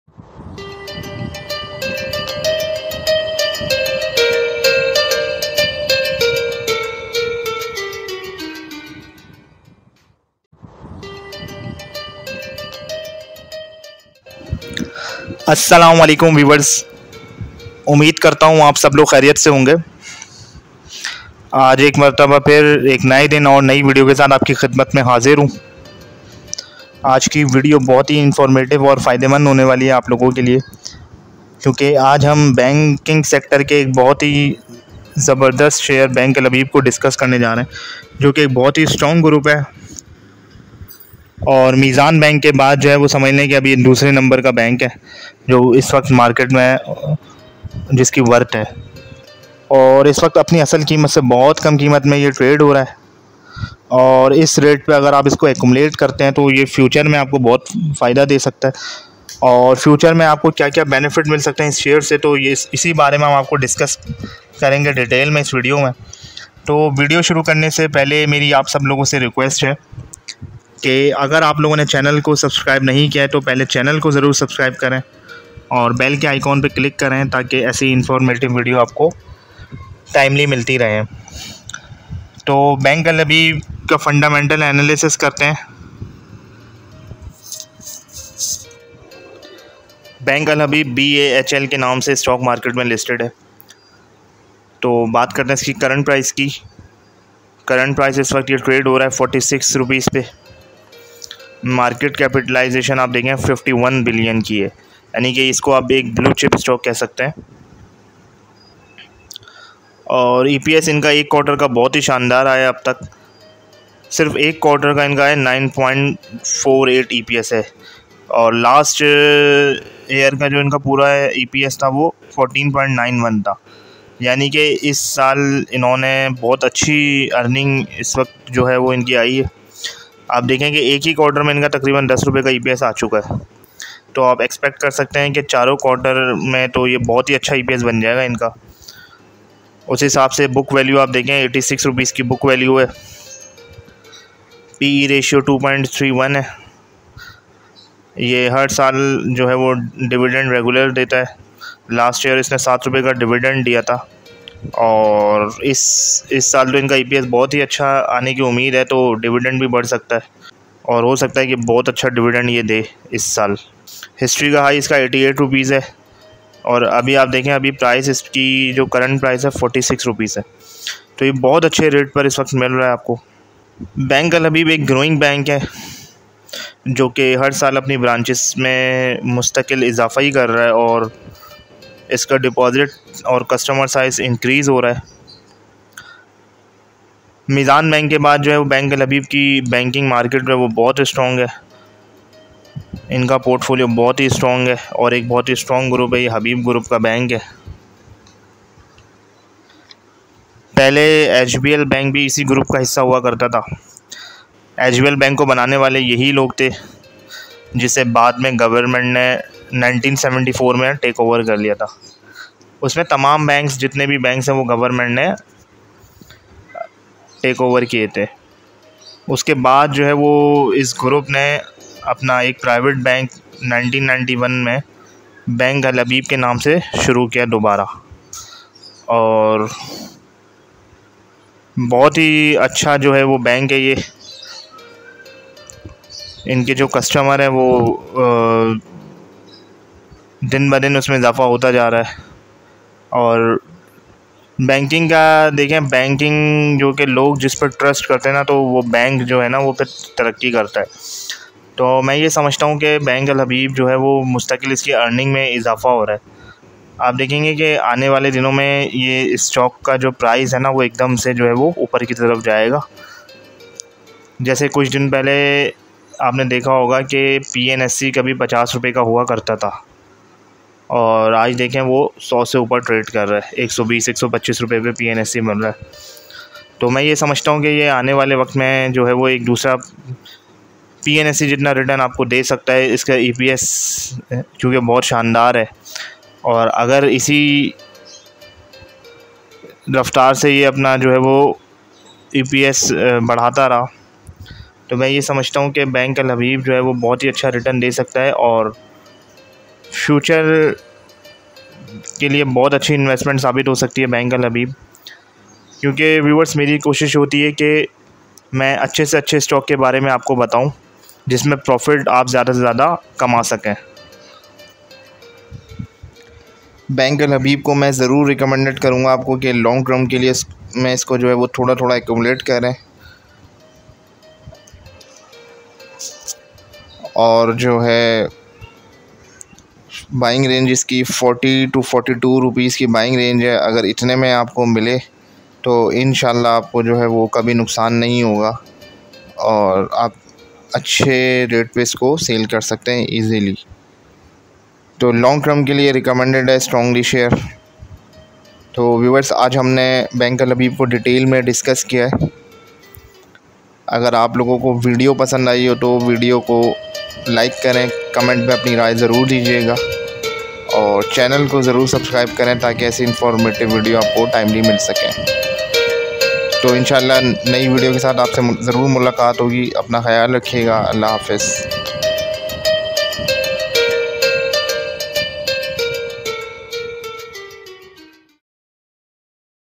Bank Al Habib Ltd | BAHL Psx | Pakistan Stock Exchange
BAHL is a growing and strong bank and very good deposit portfolio size and currently trade very low price it's a undervalued stock and now it's a opportunity for investors for long term investment

Channel Link